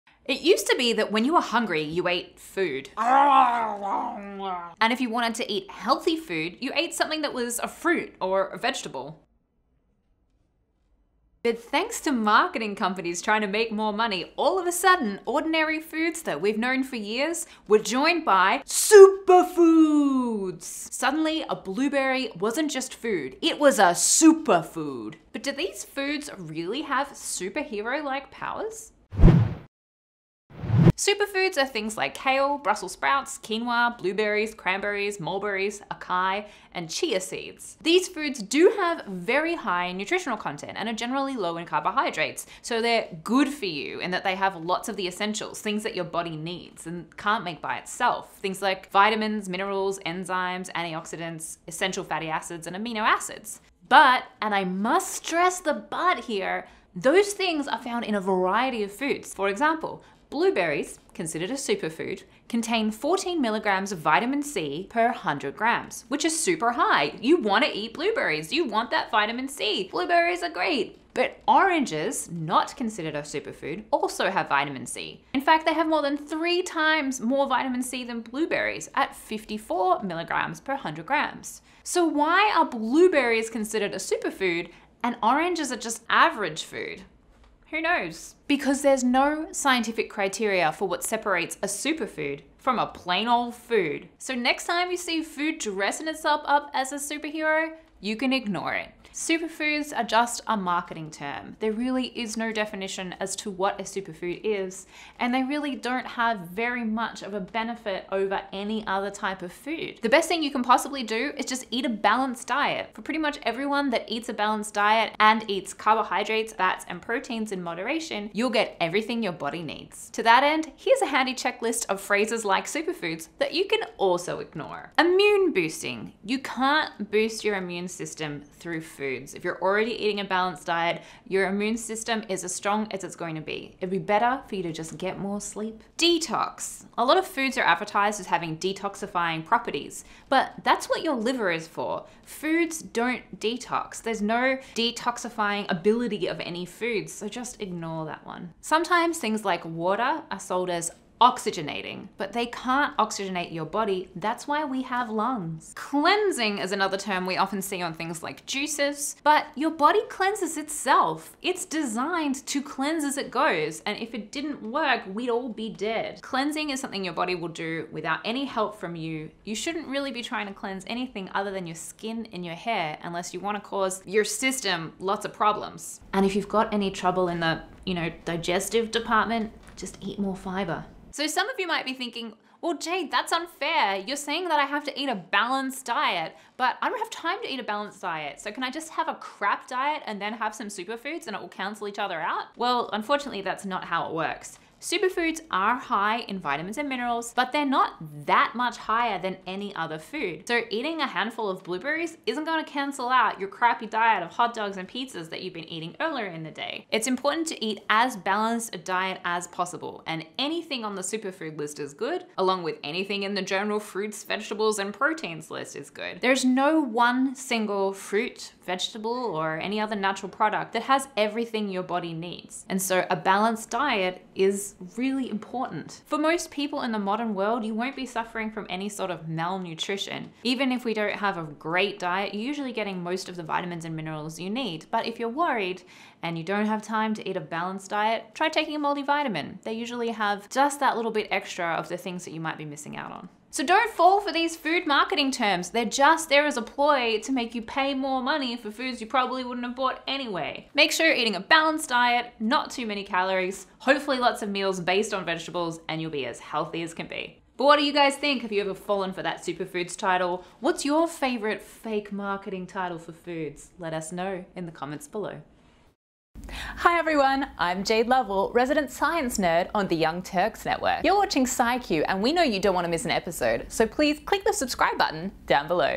'Superfood' or Super Scam? The Science Behind So-Called 'Superfoods'
It used to be that when you were hungry you ate food.

[JAYDE GOBBLING DOWN]

And if you wanted to eat healthy food, you ate something considered to be a fruit, or a vegetable.

[JAYDE HOLDING AN APPLE]

But thanks to marketing companies trying to make more money, all of a sudden, ordinary foods we’d known for years were joined by “SUPERFOODS!

Suddenly a blueberry wasn’t just food, it was a SUPERFOOD!

But do these fruits really have superhero-like powers?

[SCIQ INTRO]

“Superfoods” are things like kale, Brussels Sprouts, Quinoa, Blueberries, Cranberries, Mulberries, Acai, and Chia seeds

These foods do have a very high nutritional content, and are generally low in carbs, so they’re ‘good for you’ in that they have lots of essentials. They have the stuff your body can’t make by itself. Vitamins, Minerals, Enzymes, Antioxidants, Essential fatty acids, Amino acids.

But! And I must stress the but here! Those things are found in a variety of foods. For example, blueberries - s superfood - contain 14mg of vitamin C per 100g, which is super high. You do want that vitamin C, you need it, and so blueberries are great!

But oranges also have vitamin C. In fact, they have more than 3 times more than blueberries at 54g per 100g. So why is a blueberry super and an orange just average? Who knows!

Because There’s no scientific criteria for what separates a superfood from a plain ol food. So next time you see a food dressin itself up as a superhero, just ignore it.

Super food is just a marketing term. There really is no definition as to what a super food is and they dont have much benefit over any other type of food. The best thing you can possibly do is eat a balance diet.

To that end Here’s a handy checklist of phrases you can ignore:
 “Immune boosting” - You cant boost your immune system through food. If you are already eating a balnce diet your immune system is already as strong as its going to be. It would be better to get more sleep.

“Detox” - A lot of food is advertised as having detoxifying properties, but thats what your liver is for. Foods dont detox

“Oxygenating” - Sometimes things like water is sold as oxygenating, but they cant oxygenate your body thats why we have lungs

 “Cleansing” - is another term we often see in juices but your body cleanses itself. its designed to cleanse as it goes. and if it didn't work we would all be dead. you shouldn’t be trying to cleanse anything other than your skin or hair.

So some of you might be thinking, well Jayde thats unfair, you're saying I have to eat a balance diet but I dont have time to eat a balance diet, so can I just have a crap diet and then have some super food and itll cancel  eachother out?

well unfortunatly thats not how it works, superfoods are high in vitamins and minerals but they are not that much higher than any other food. So eating a handful of blueberries isnt gonna cancel out your crappy diet of hotdogs and pizza that you've been eating earlier in the day. Its important to eat a balance diet as possible. There is no one fruit or vegetable that has everything your body needs, so a balance diet is really important.

So dont fall for these food marketing terms, there just there for you to pay more money for foods you probably wouldnt have bought anyway.

This video is presented by Jayde Lovell, Directed by Yohana Yoshe, and Edited by Sarita Liu at Youtube Space NY. Script by Emelye Lovell.